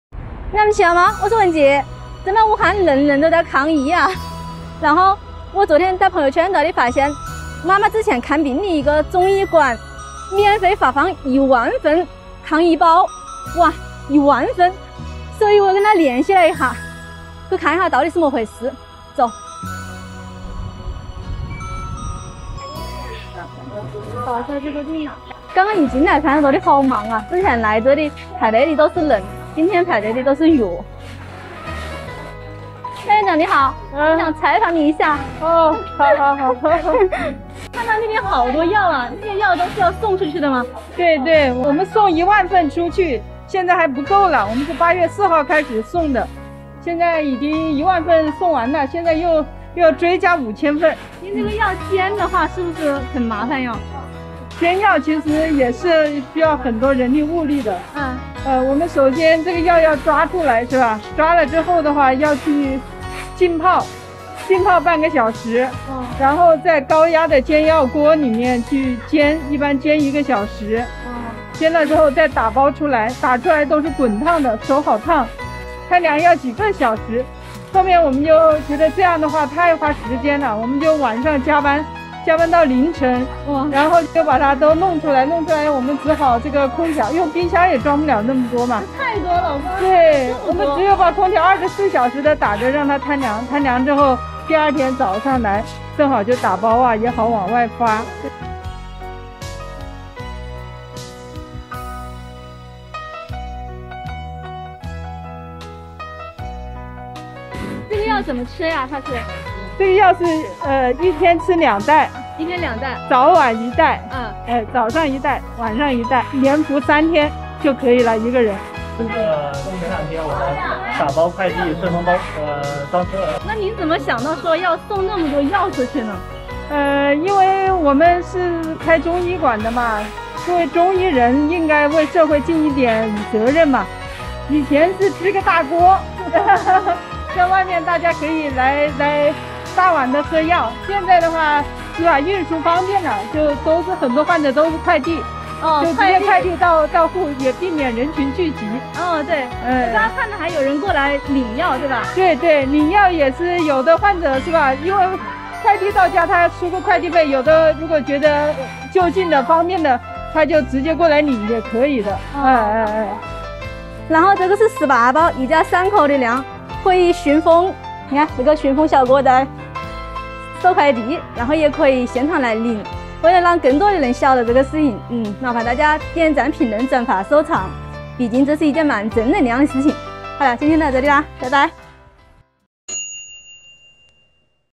正能量：武汉这家中医馆，免费送10000份抗疫包，可邮寄速速去领
[武汉雯婕] 正能量：武汉这家中医馆，免费送10000份抗疫包，可邮寄速速去领

记录武汉生活的美好！一个爱吃吃、会省钱的女子!